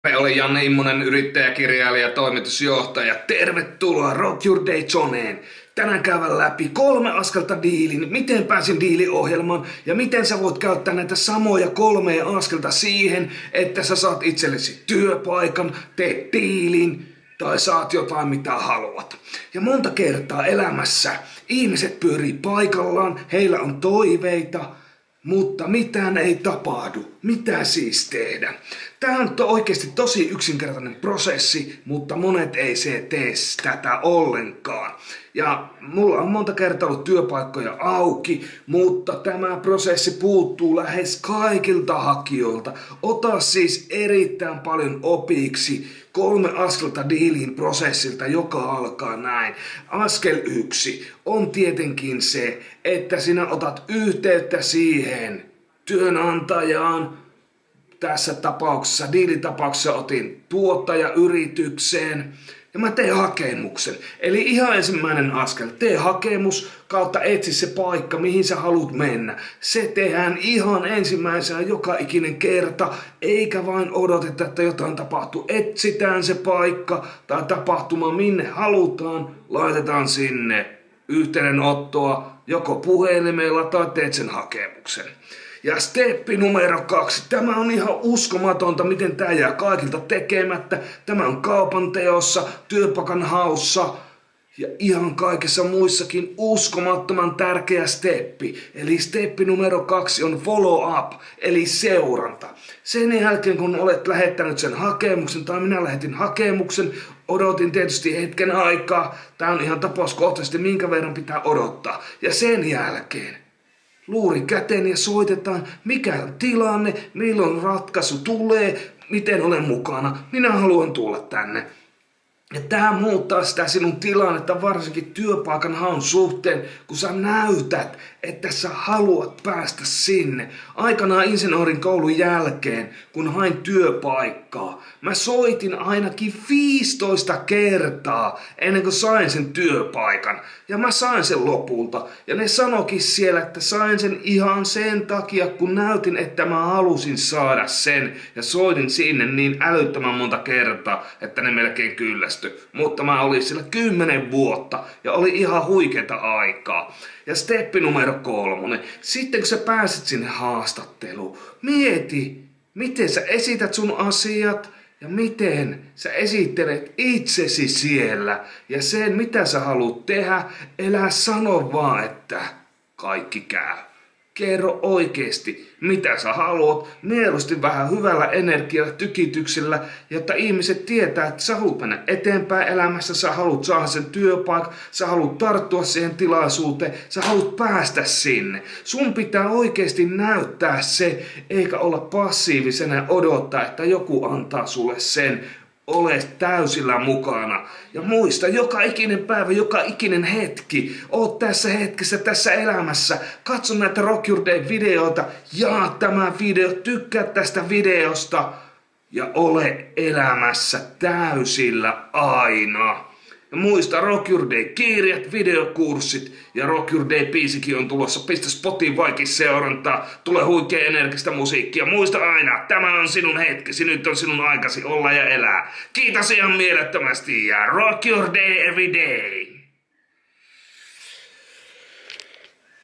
Rock Your Day: 3 Askelta Diiliin- Näin Pääsin mukaan diili ohjelmaan
UUTUUS VIDEOKURSSI: 3 Askelta Menestyä Työssäsi

YHTEENVETO:

Tällä videolla kerron 3.askeleeen prosessin, jolla pääsin Diili ohjelmaan mukaan. Voit käyttää tätä samaa prosessia diilin tekemiseen tai vaikka ihan työpaikan hakemiseen.